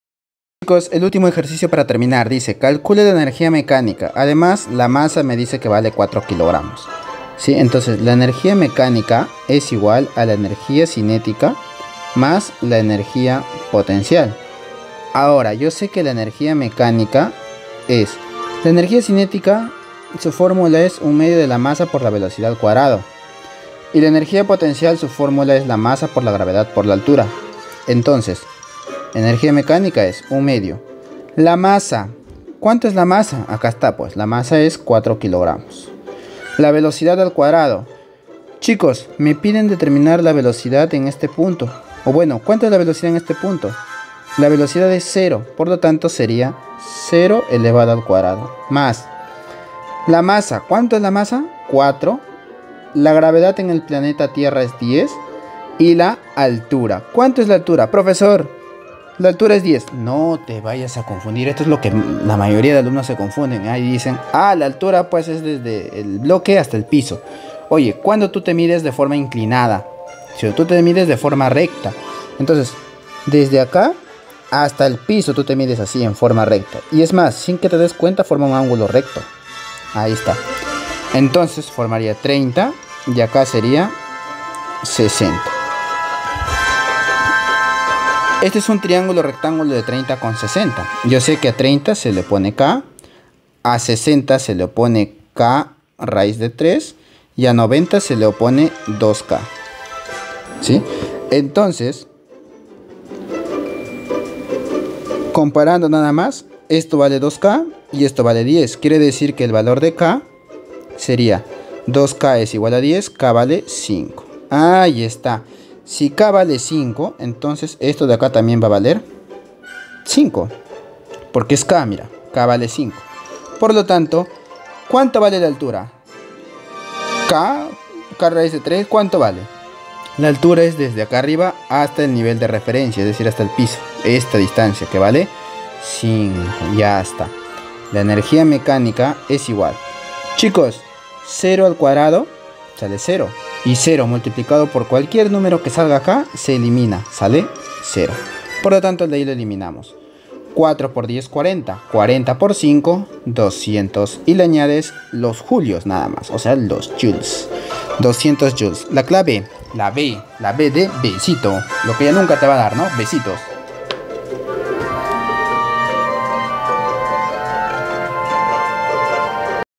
Calcular la energía mecánica m = 4kg | ENERGÍA MECÁNICA, CINÉTICA y POTENCIA
TEMA: ENERGÍA MECÁNICA - ENERGÍA CINÉTICA - ENERGÍA POTENCIAL
CURSO: FÍSICA
Ejercicios resueltos de CUARTO de Secundaria 4° sec

15. Calcular la energía mecánica m = 4kg}
a) 100 J
b) 200
c) 300
d) 400
e) 500

¡Descubre los secretos de la energía mecánica!
En este video, exploraremos el fascinante mundo de la energía mecánica, aprendiendo:

¿Qué es la energía mecánica? Descifraremos su definición, tipos y cómo se transforma de una forma a otra.
Cálculo de la energía cinética y potencial. Aprenderás a calcular la energía cinética y potencial gravitatoria de diversos objetos.
Conservación de la energía mecánica. Demostraremos el principio de conservación de la energía mecánica en diferentes situaciones.
Ejemplos prácticos y aplicaciones. Observa cómo la energía mecánica se manifiesta en el mundo real, desde el movimiento de un péndulo hasta el funcionamiento de una montaña rusa.
Este video es ideal para:

Estudiantes de física de bachillerato y universidad que buscan una comprensión profunda de la energía mecánica.
Profesores de física que desean recursos dinámicos y atractivos para sus clases.
Cualquier persona curiosa por el mundo de la física y su relación con la energía mecánica.
No te pierdas este viaje al corazón de la física y aprende sobre la energía mecánica de una manera divertida e intuitiva!